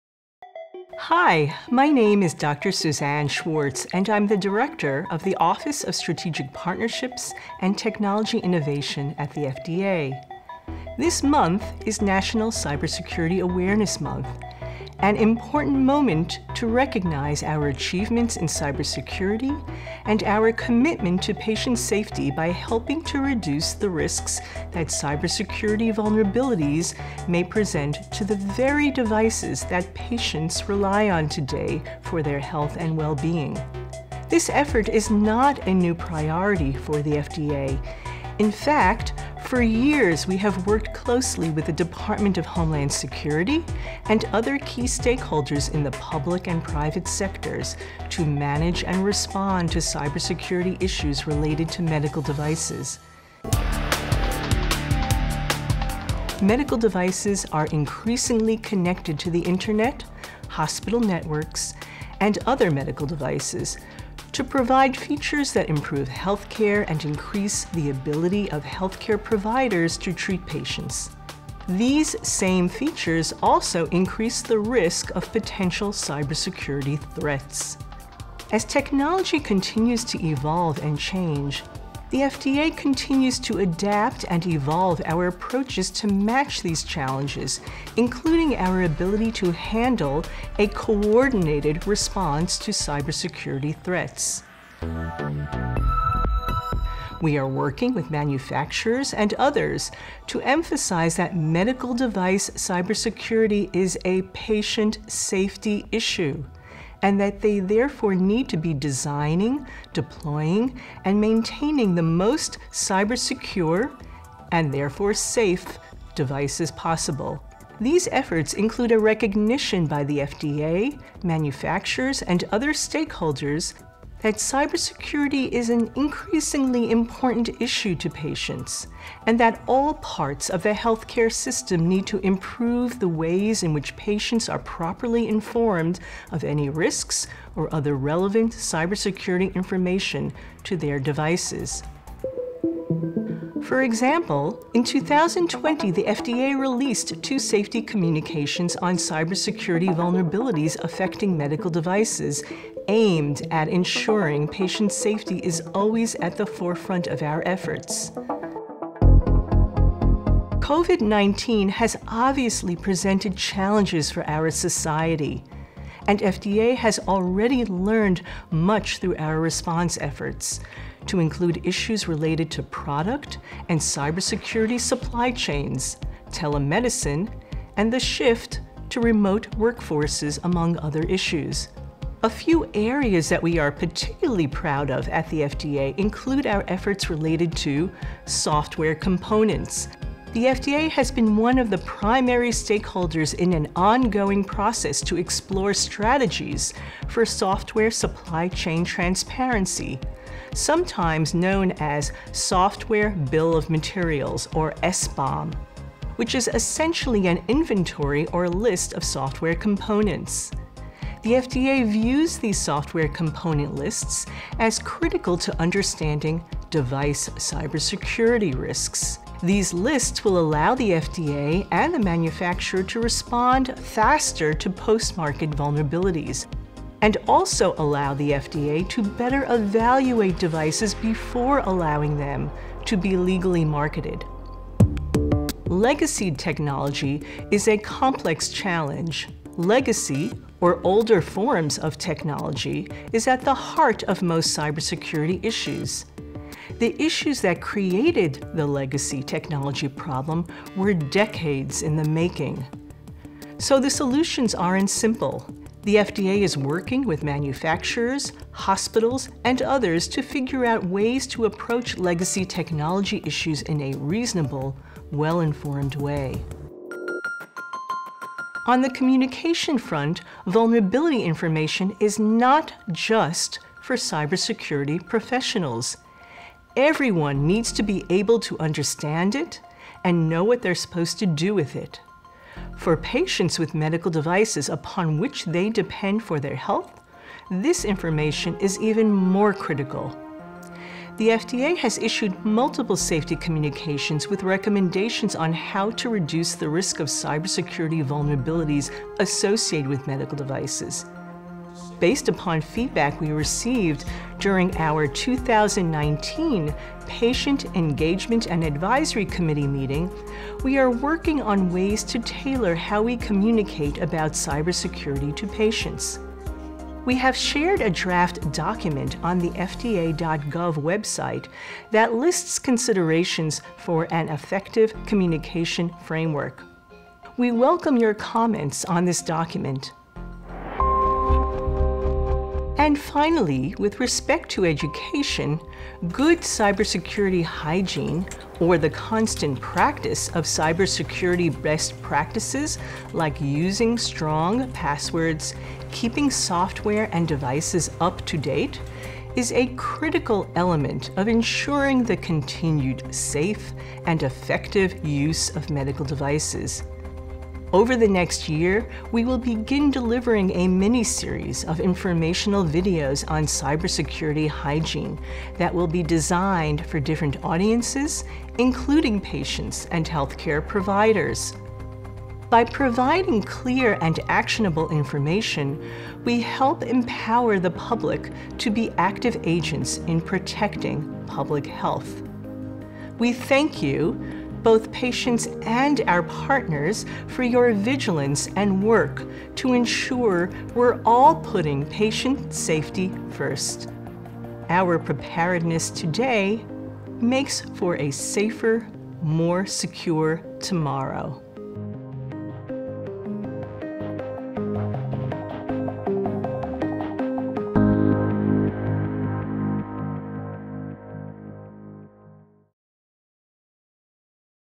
October is National Cybersecurity Awareness Month
Medical devices are increasingly connected to the Internet, hospital networks, and other medical devices to provide features that improve health care and increase the ability of health care providers to treat patients. These same features also increase the risk of potential cybersecurity threats.

To help reduce these risks, the FDA works with medical device manufacturers, the Department of Homeland Security, and other stakeholders to emphasize that medical device cybersecurity is a patient safety issue, and that they therefore need to be designing, deploying, and maintaining the most cybersecure—and therefore safe—devices possible.